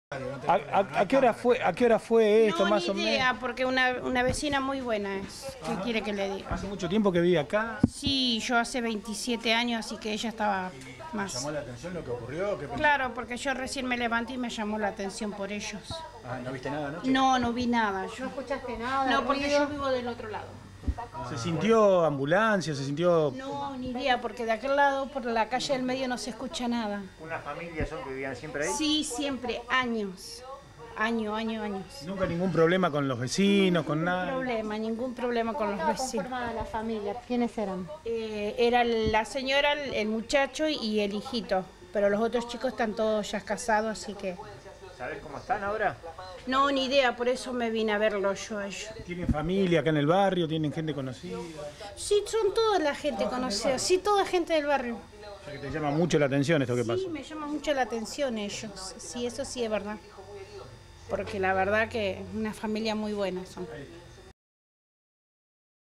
Prendieron fuego a una familia. Tres personas con graves quemaduras. Calle 1849 al 6600
TRES PERSONAS INTERNADAS CON GRAVES QUEMADURAS
MARY – VECINA

Un hombre se encuentra en estado desesperante en el Hospital de Emergencias Clemente Alvarez (Heca) debido a las graves quemaduras sufridas en la mayor parte de su cuerpo, luego de que desconocidos ingresaran a su domicilio y le prendieran fuego a la vivienda, en la zona oeste de Rosario. Su esposa, en tanto, sufrió también quemaduras mientras que un hijo adolescente de la pareja fue derivado al Hospital Eva Perón con quemaduras en sus piernas. dos personas que aparentemente se movilizaban en una moto ingresaron y prendieron fuego a una habitación donde descansaban tres miembros de una familia.